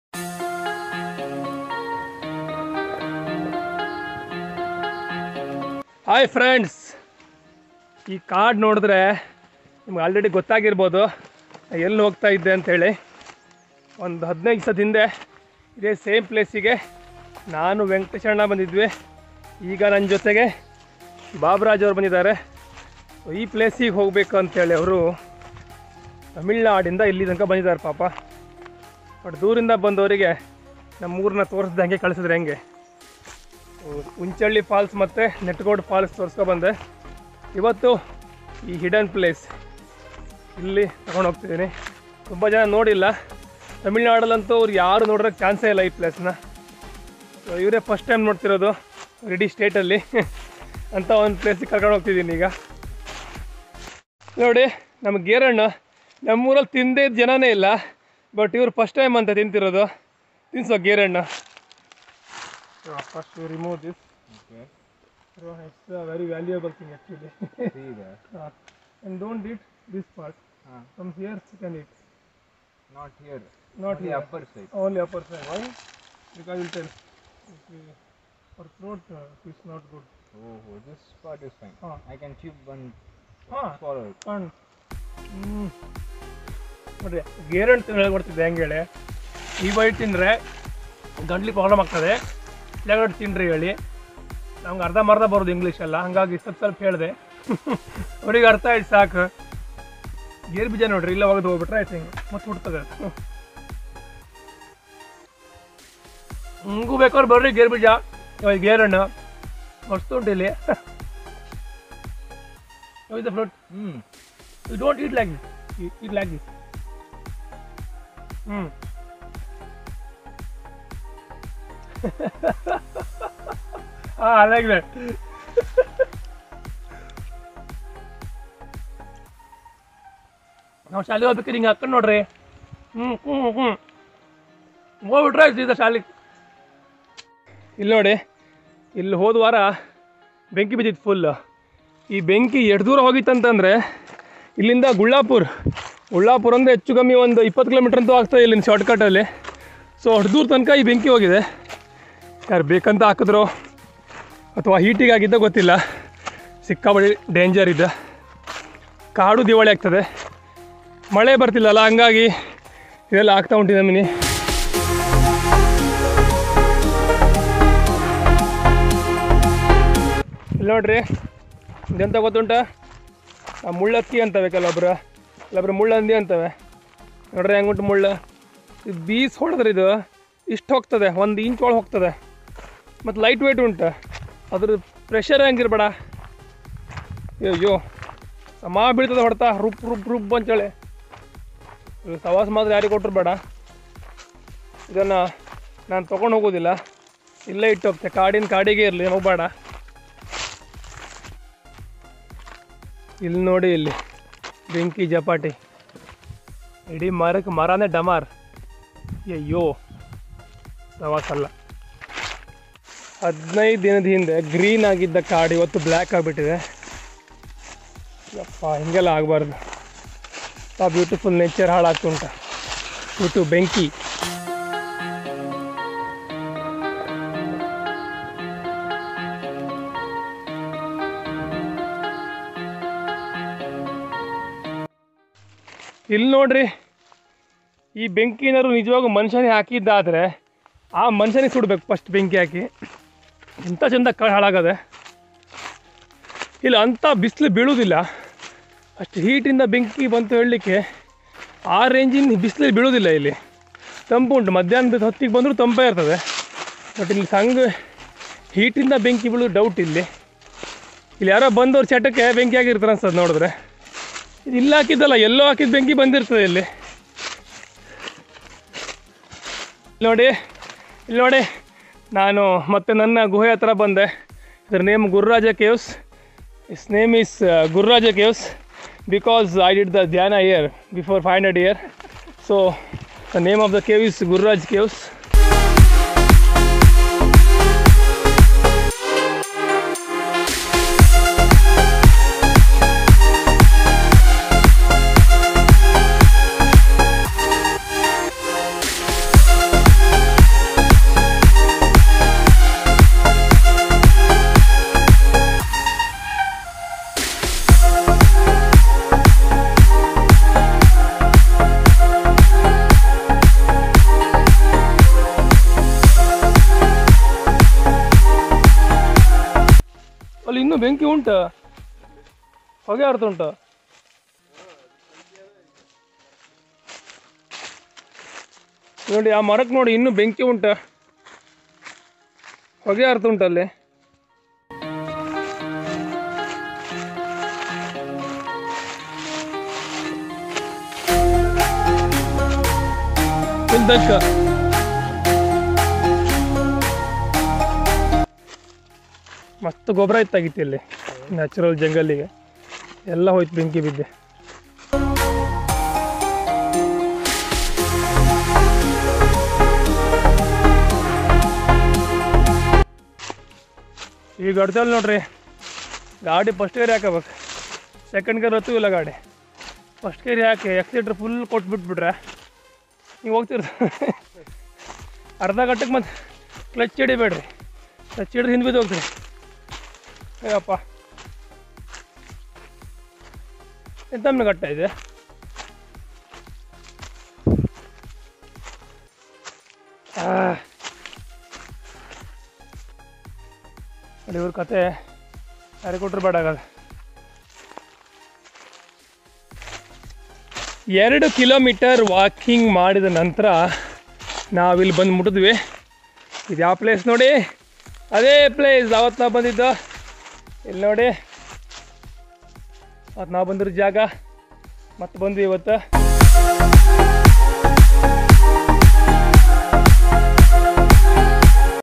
Forest fire vs Fruits | ಕಾಡಿಗೆ ಬೆಂಕಿ ಬಿದ್ದರು ಹಣ್ಣಿಗೆ ಎನು ಆಗಿಲ್ಲ
Forest fire vs Fruits
Natural fruits are still safe
ಕಾಡಿಗೆ ಬೆಂಕಿ ಬಿದ್ದರು ಹಣ್ಣಿಗೆ ಎನು ಆಗಿಲ್ಲ
Trekking to hidden place